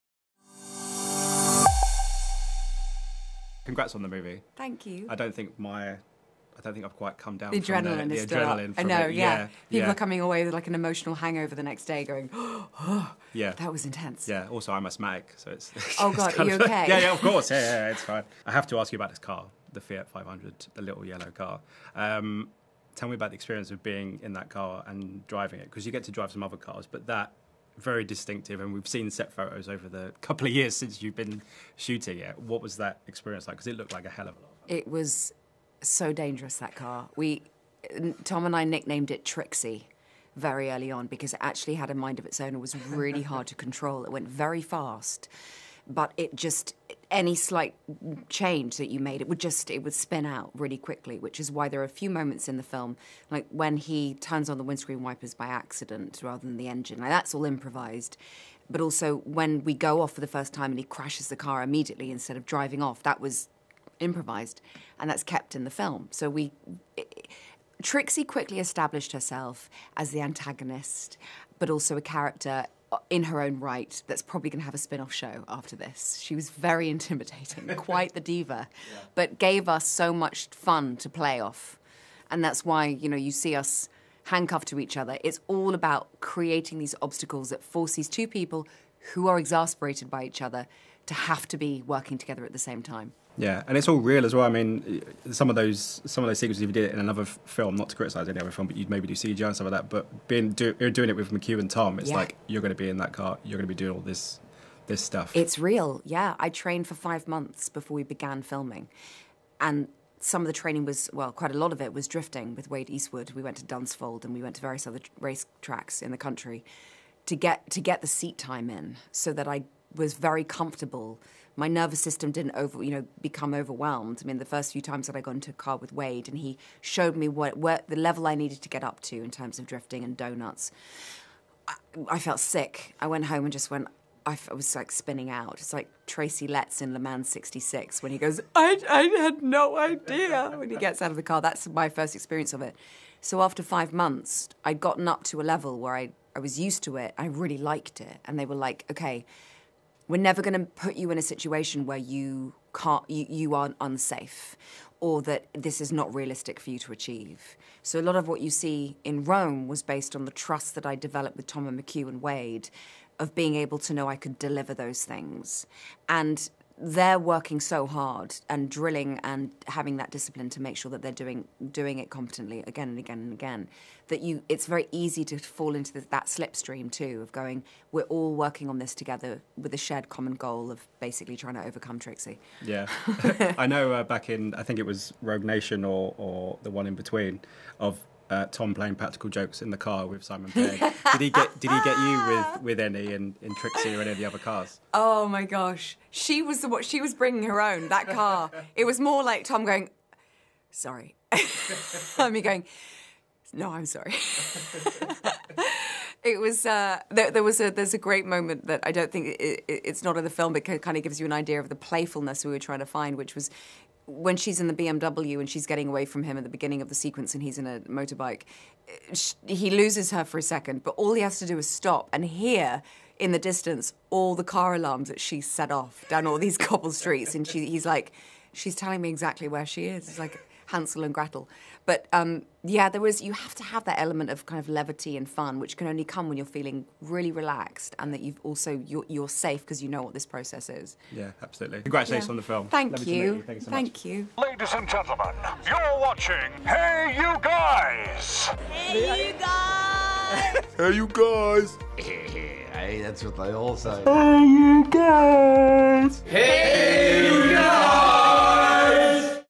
Hayley Atwell - Mission: Impossible Dead Reckoning Part 1, her intense driving training & 'Trixie'
Plot: Ethan Hunt (Tom Cruise) and his IMF team embark on their most dangerous mission yet: To track down a terrifying new weapon that threatens all of humanity before it falls into the wrong hands. With control of the future and the fate of the world at stake, and dark forces from Ethan’s past closing in, a deadly race around the globe begins. Confronted by a mysterious, all-powerful enemy, Ethan is forced to consider that nothing can matter more than his mission – not even the lives of those he cares about most.

The movie hits cinemas on July 11th.